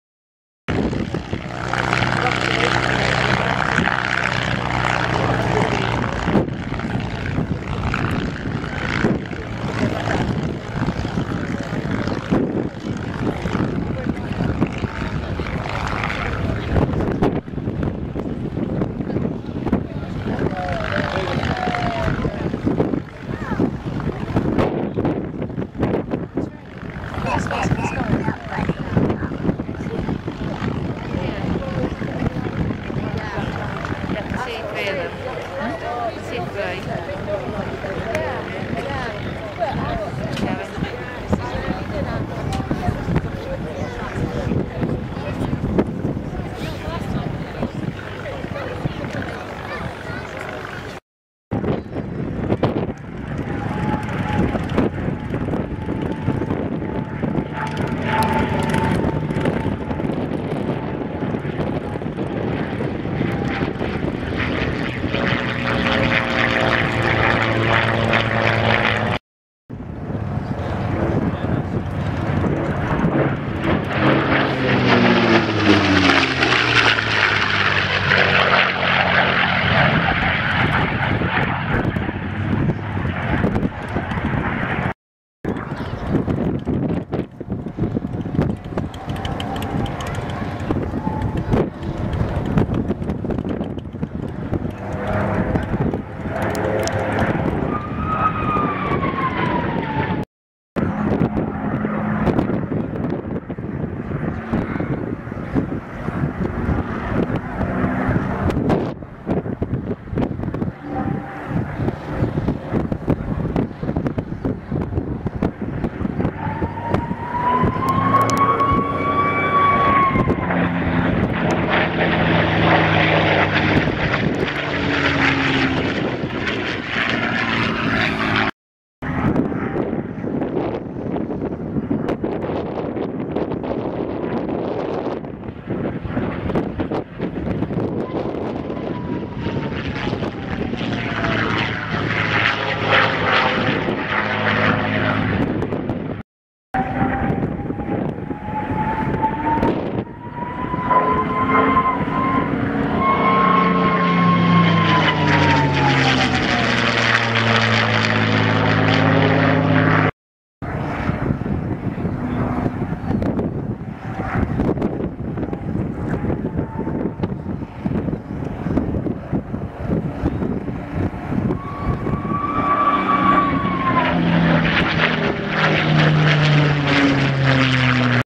ferocious frankie
p51 mustang 'ferocious frankie' showing her paces at Merlin Pageant,Hucknall air display,Rolls Royce,watnall,many thanks to all for a great day out.